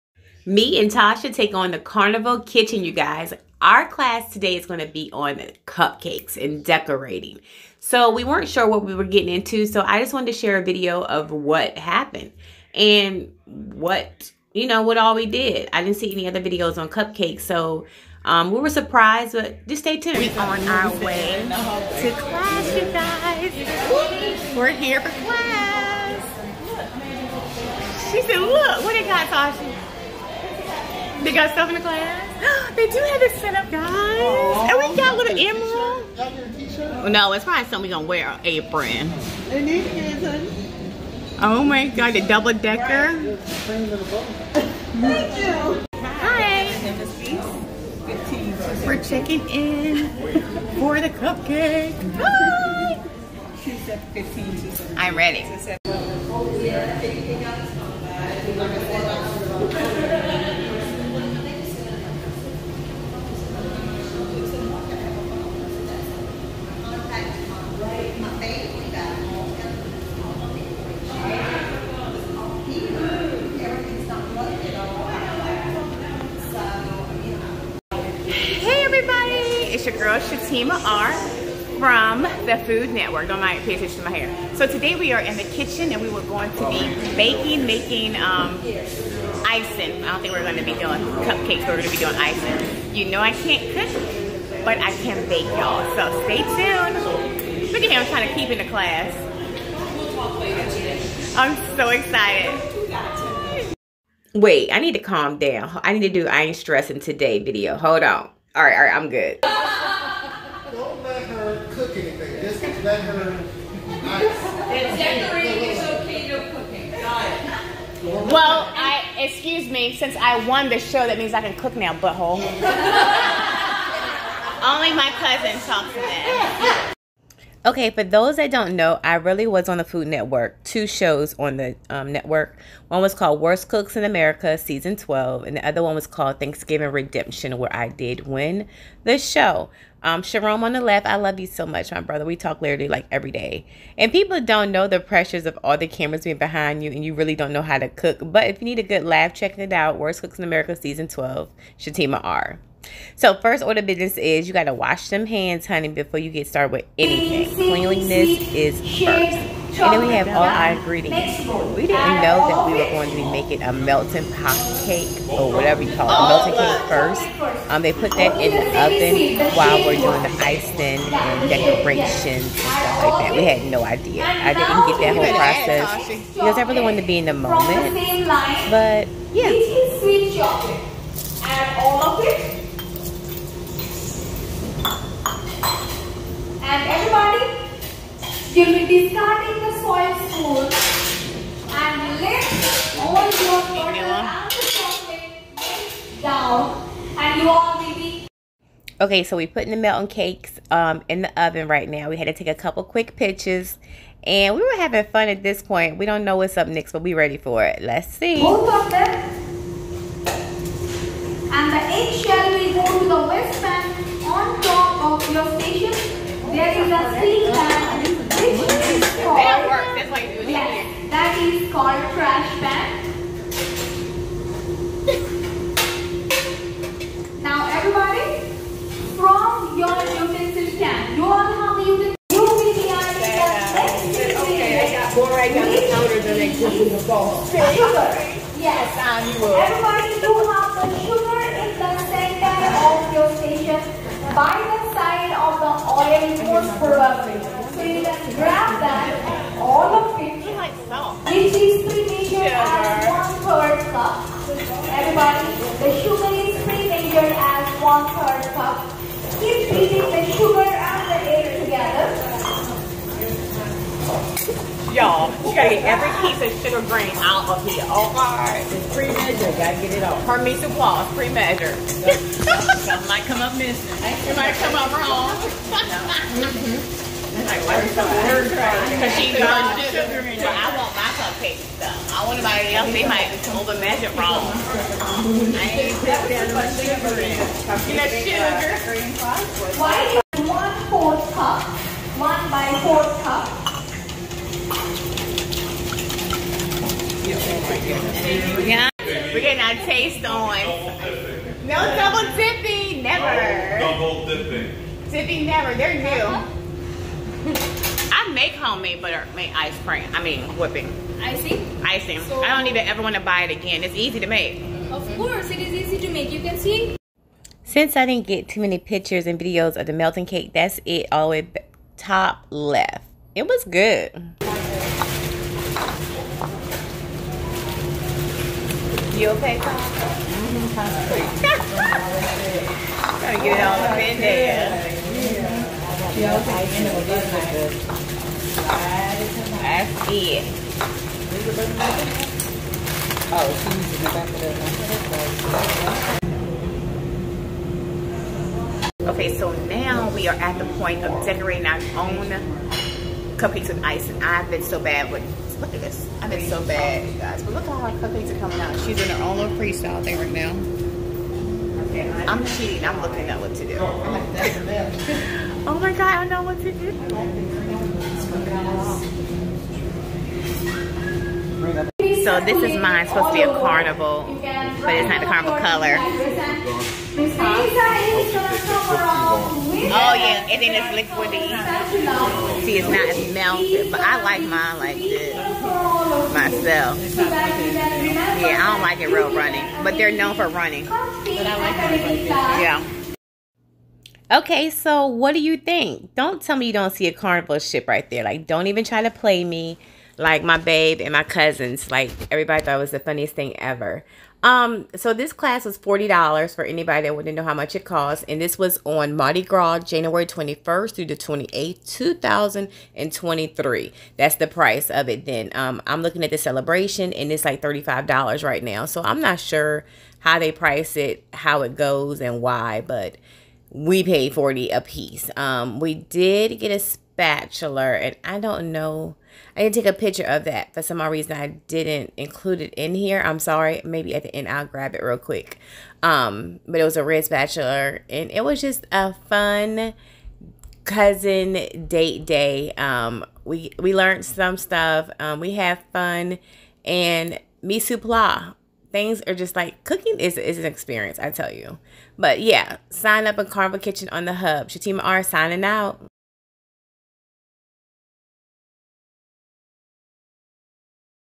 Carnival Kitchen 🧁 Class
Time to make some cupcakes and decorate them very pretty or at least the carnival way so let's see what they're talking about also we had a surprise during class yeah stay tuned and sign up for the class it's just like something to try once I'm thinking about maybe trying a few more of the other classes..

** Put me in your highest Quality for Best Experience**

Pre hotel: Spring Hill suites airport
Transportation: Carnival Shuttle

Carnival Mardi Gras Birthday Cruise is in FULL Effect!!! Jan 21-28, 2023